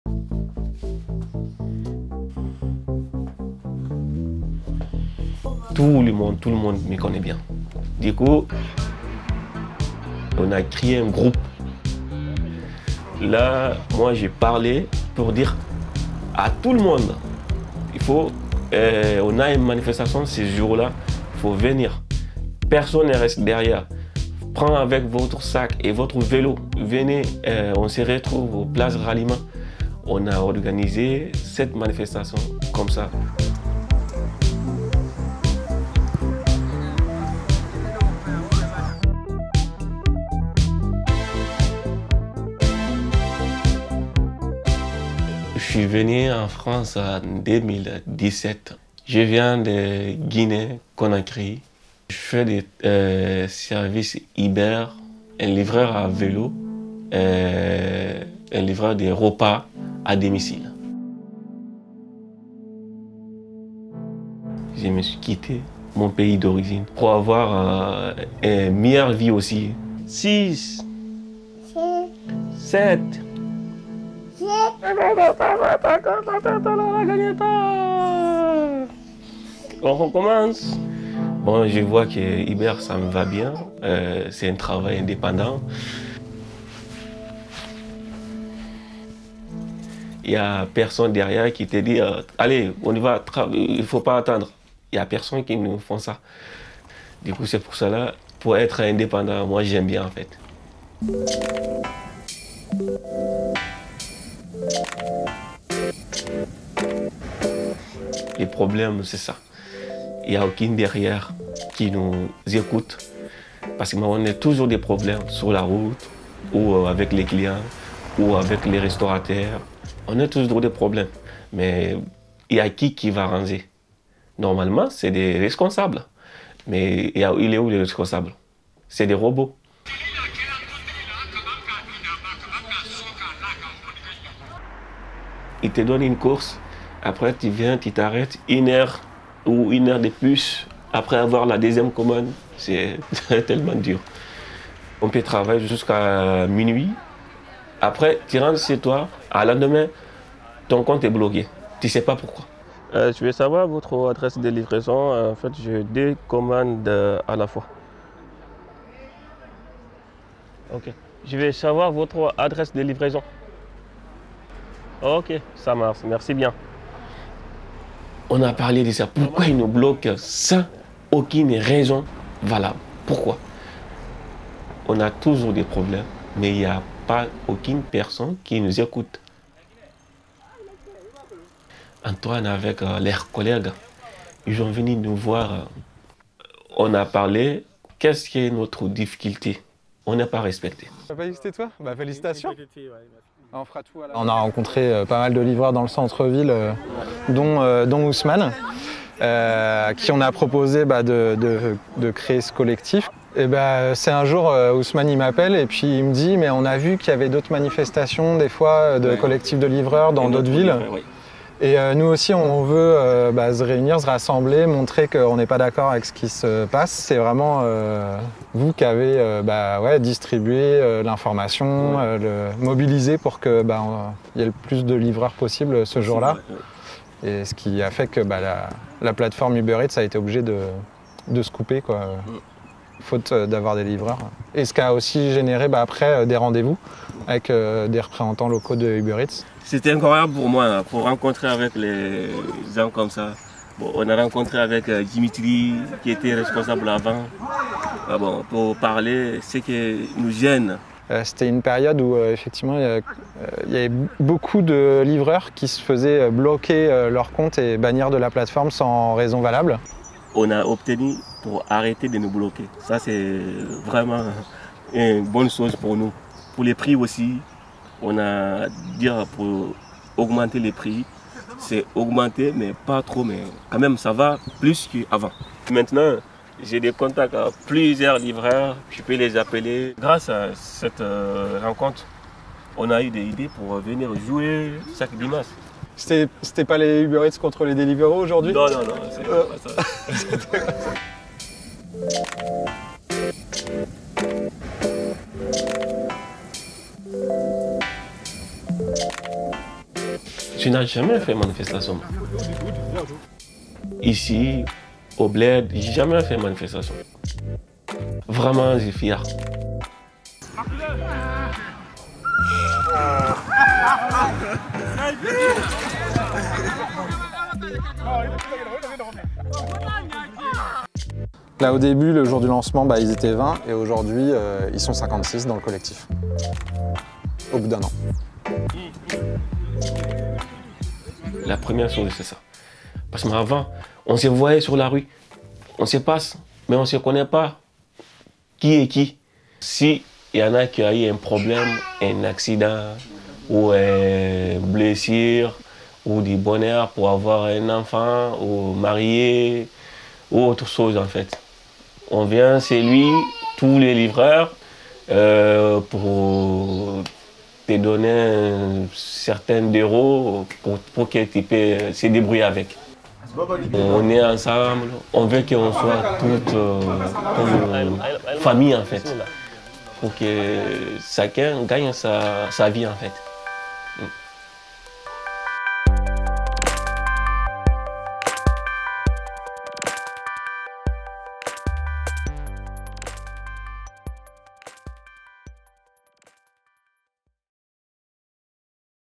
Parlons Engagement : Ousmane, engagé dans un collectif de livreurs indépendants
L'engagement, pour vous, c'est quoi ? Participez à notre grande enquête